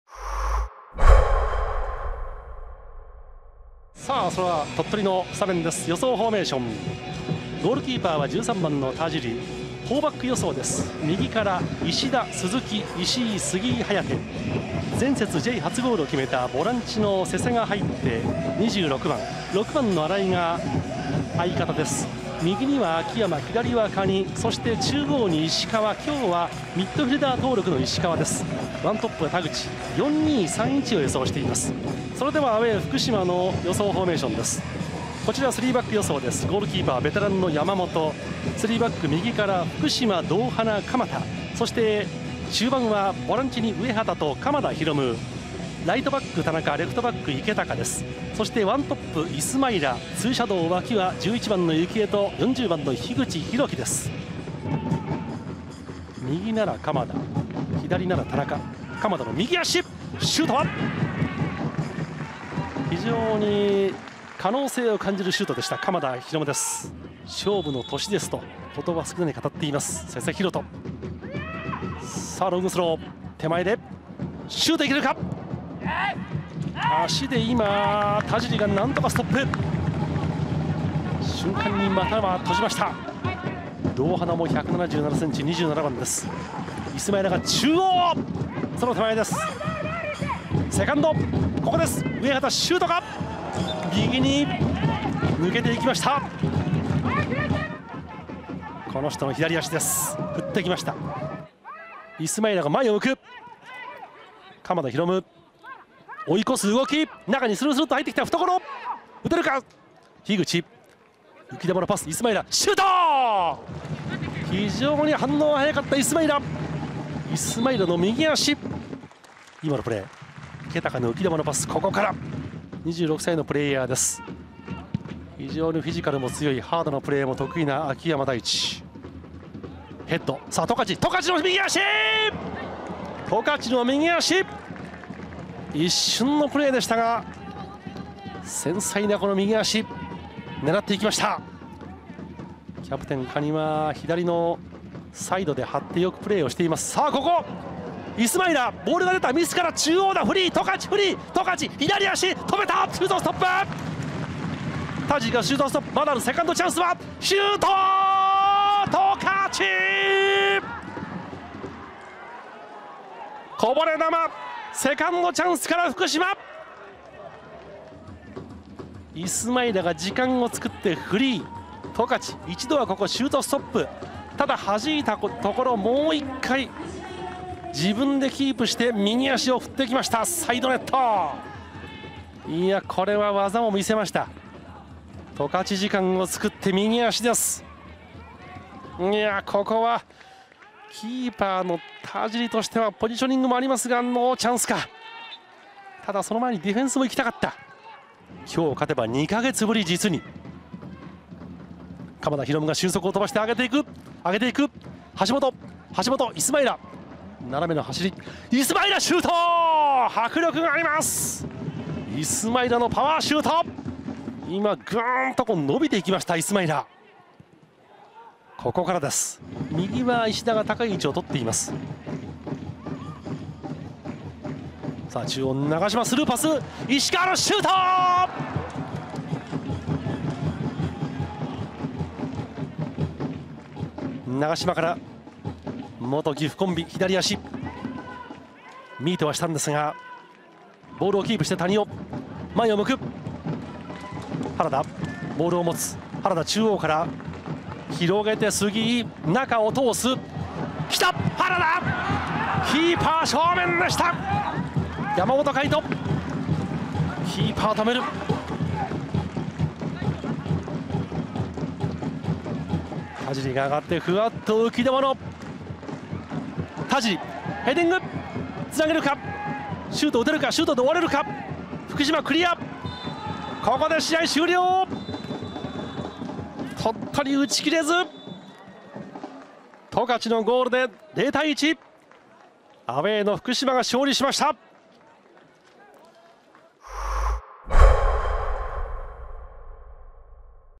ハイライト：2021明治安田生命J3リーグ第13節 ガイナーレ鳥取 0-1 福島ユナイテッドFC
2021明治安田生命J3リーグ第13節
ガイナーレ鳥取 0-1 福島ユナイテッドFC

【得点者】
65分　トカチ（福島）